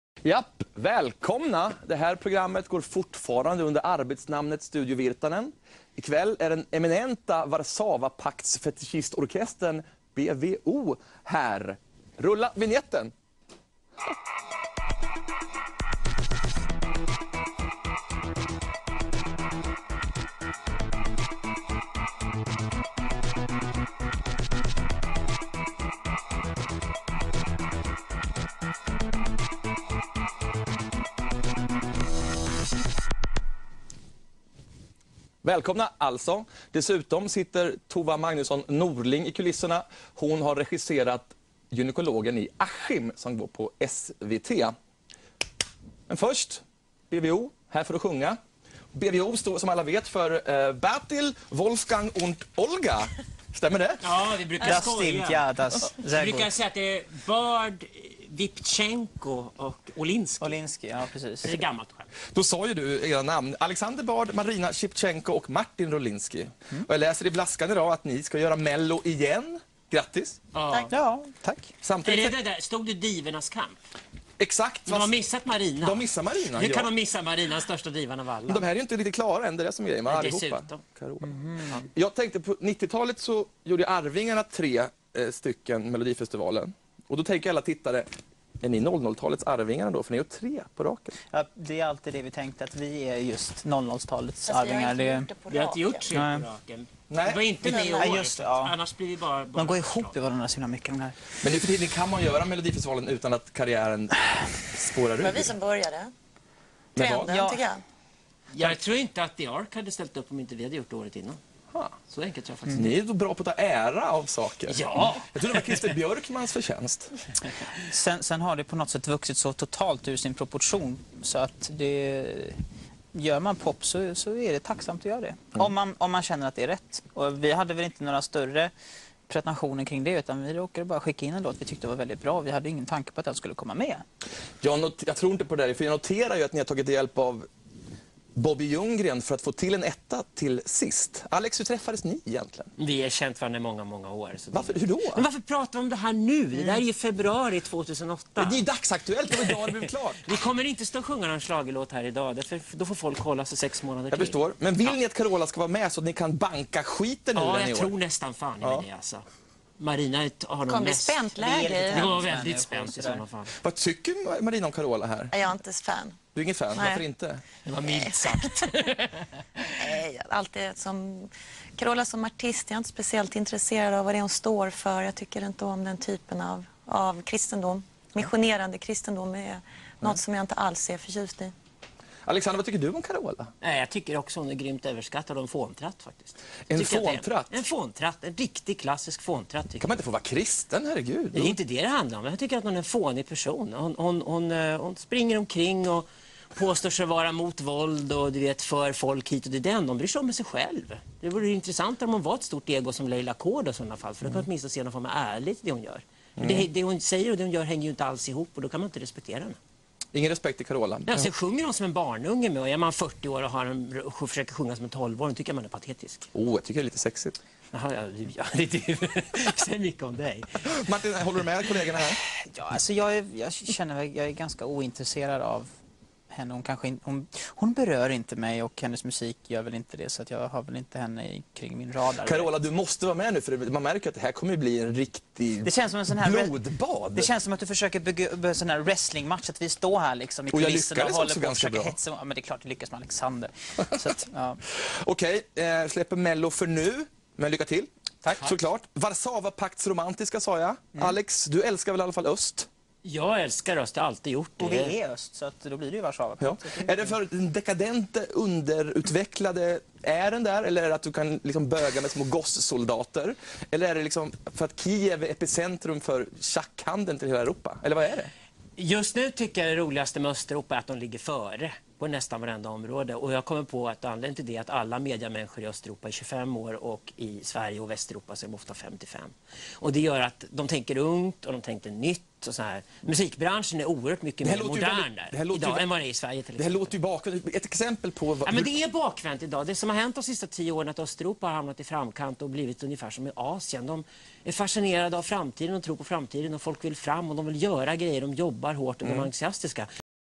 BWO Studio Virtanen Del 1 "Carola är en fåntratt!"
Virtanen intervjuar BWO. Kolla in min blogg